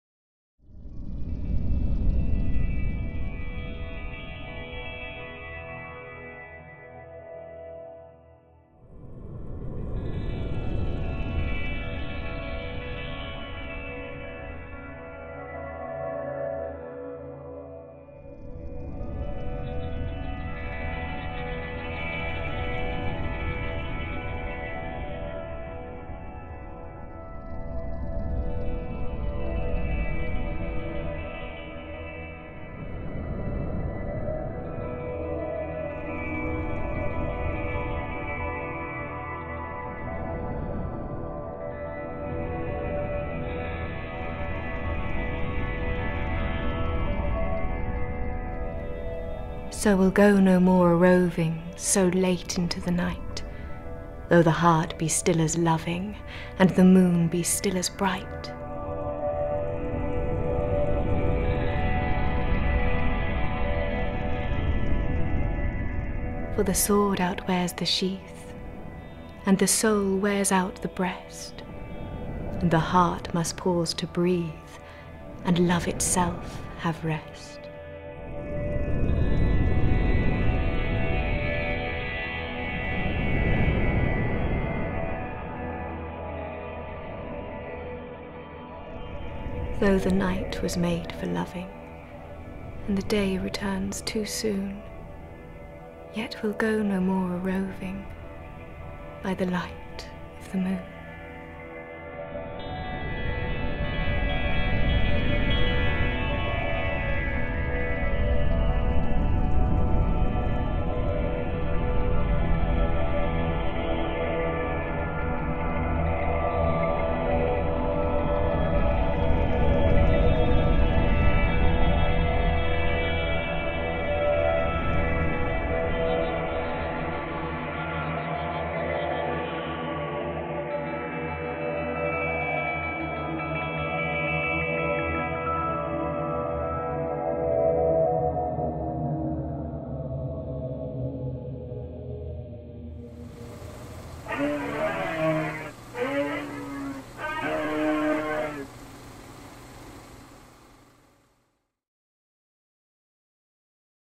Bite: No More A-Roving (Vampire Mix)
No More A-Roving (1817) By Byron
Directed by Kevin Jackson
Camera Spike Geilinger
Editing and Music Colin Minchin
Poem read by Pamela Banks